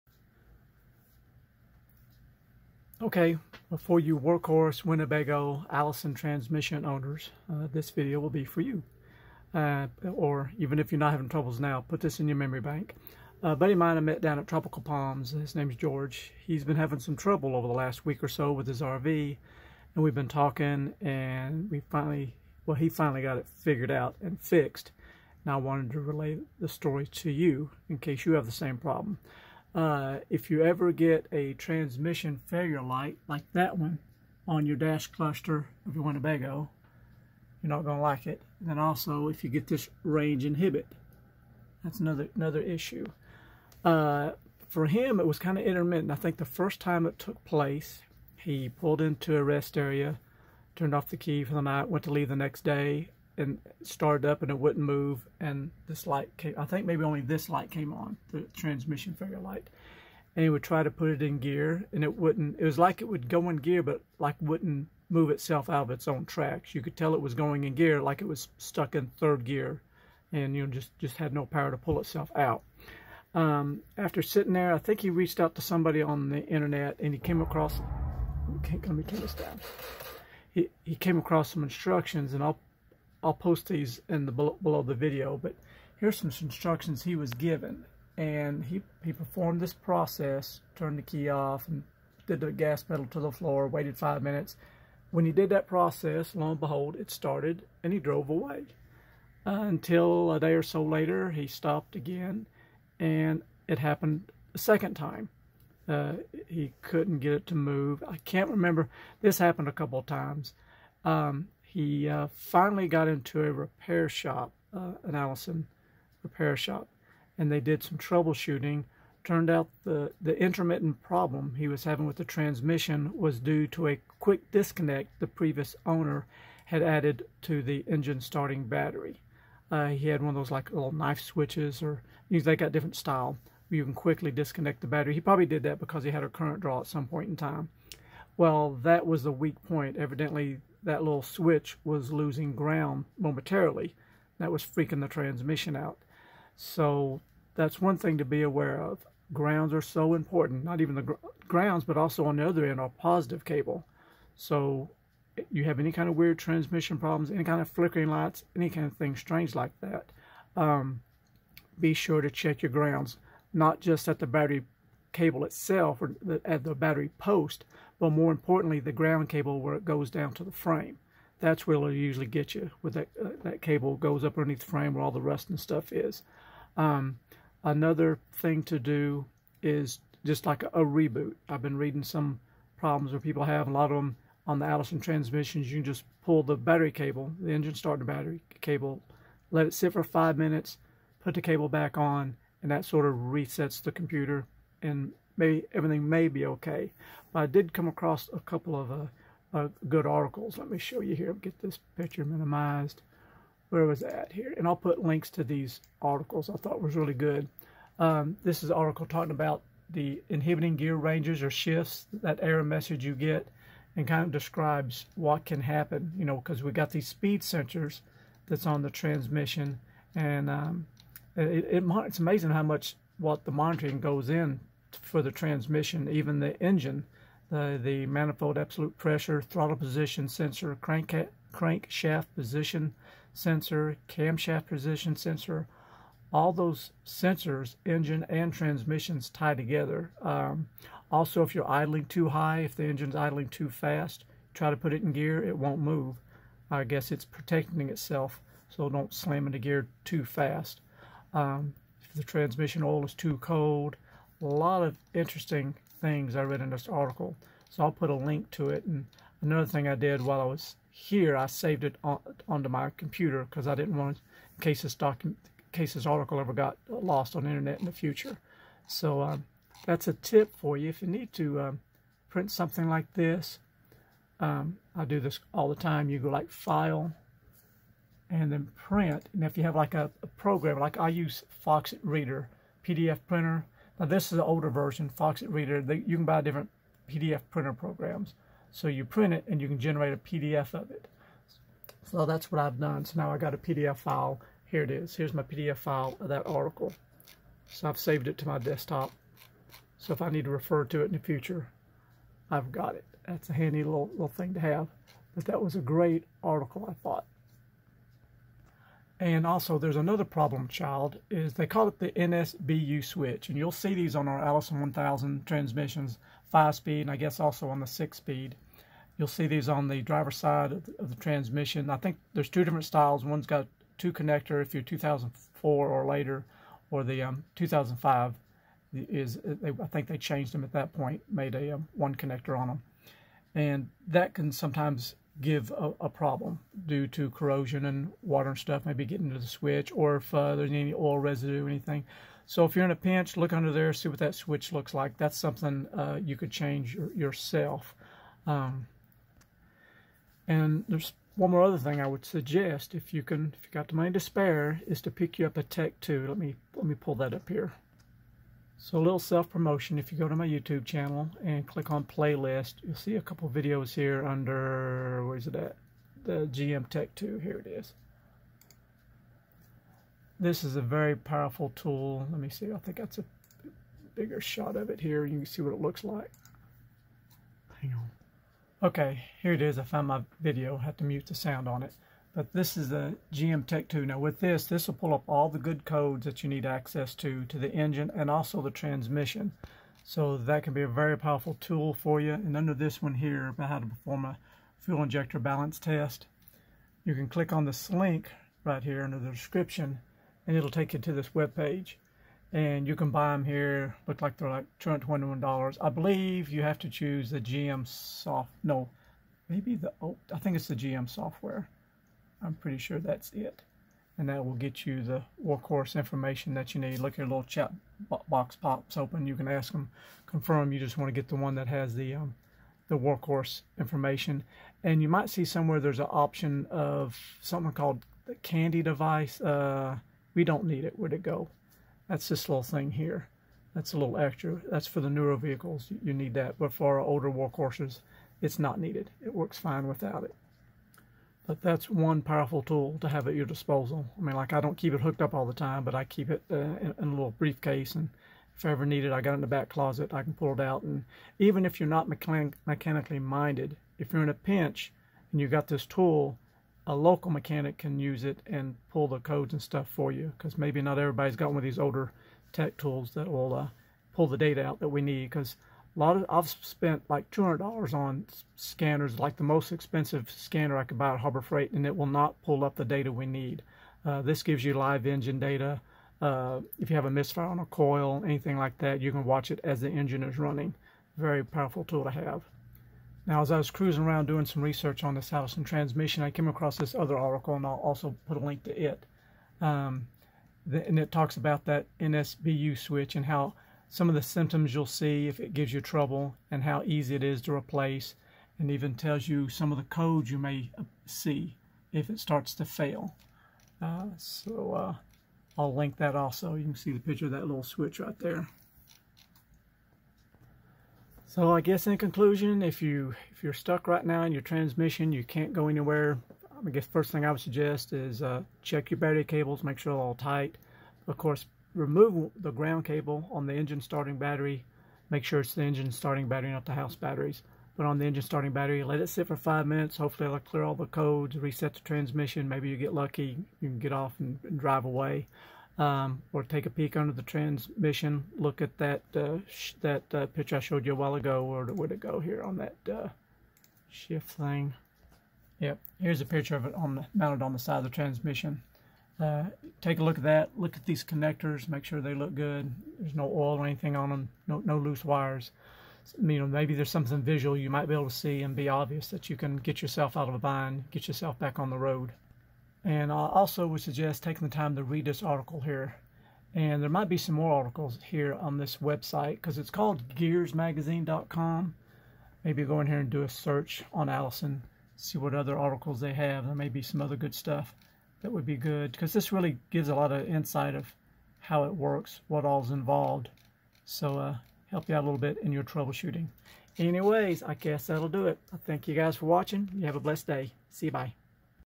Workhorse Allison Transmission Range Inhibit Dash Light is On Along with Gear Light RV Won't Move
Workhorse Allison Transmission Range Inhibit Dash Light is On Along with Gear Light RV Won't Move and you stuck where you are. This may help you do some trouble shooting of your own This applies to the Allison LCT1000 or Allison 1000 and Allison 2100MH
How to reset a transmission control module Chevy.. For Allison? Not sure but worth a try
Step 1: Turn-key position.
The first thing you want to do is place your key in the ignition
and turn it twice so all of the lights on the dashboard illuminate. ...
Step 2: Foot on the gas pedal to the floor. ...
Step 3: Turn key off and release gas pedal. ...
Step 4: Wait 5 minutes
Step 5: Ready to drive.??
Next disconnect engine battery for 5 minutes to see if that clears the error code so you can drive away.